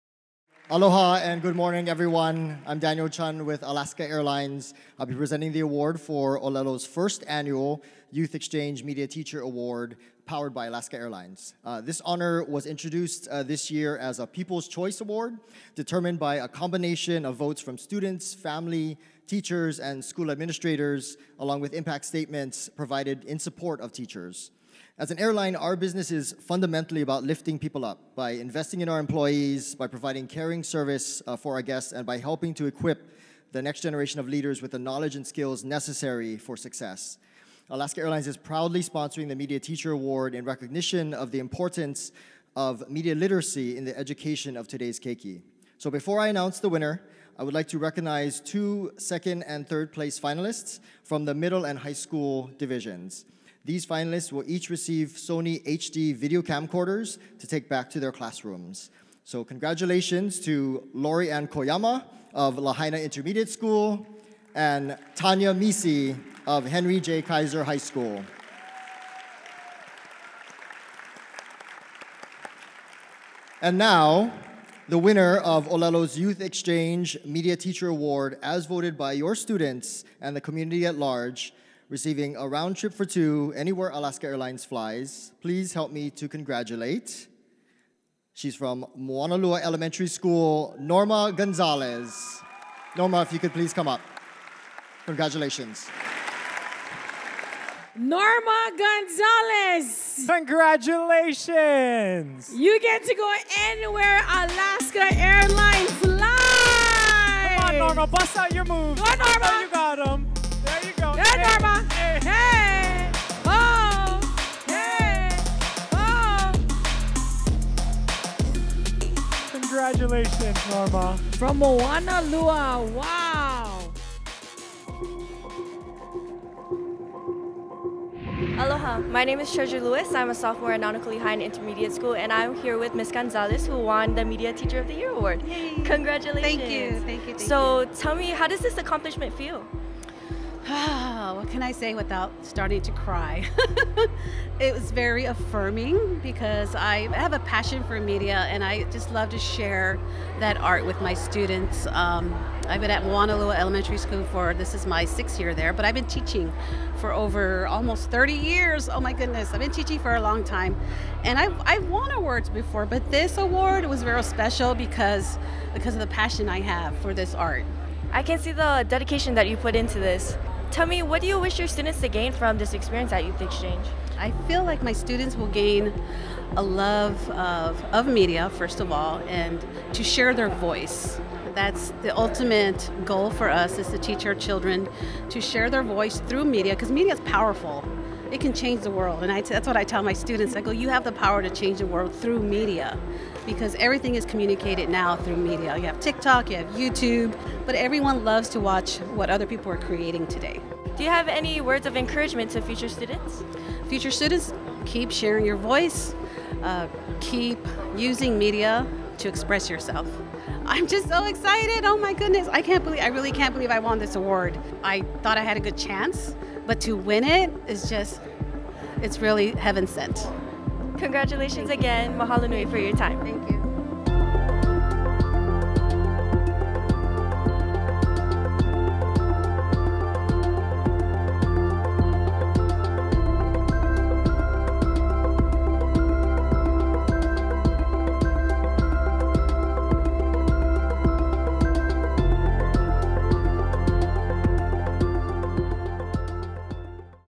YXC Media Teacher of the Year 2022 | Norma Gonzalez
ʻŌlelo Community Media presented our first annual YXC Media Teacher Award, powered by @AlaskaAirlines . The honor was introduced this year as a "people's choice" award determined by a combination of votes from students, family, teachers, and school administrators; along with impact statements provided in support of teachers.

Alaska Airlines, a company dedicated to lifting people up, proudly sponsored the Media Teacher Award in recognition of the importance of media literacy in the education of today’s keiki. The teacher with the most votes and the most impactful stories wins a round trip for 2 to any destination served by Alaska Airlines and was be recognized on this year’s live broadcast of the Youth Xchange Awards Wednesday, April 20, 2022.

The winner of ʻŌlelo’s YXC Media Teacher Award as voted by her students and the community at large was Norma Gonzalez, Moanalua Elementary School.

Read what the community had to say about Ms. Gonzalez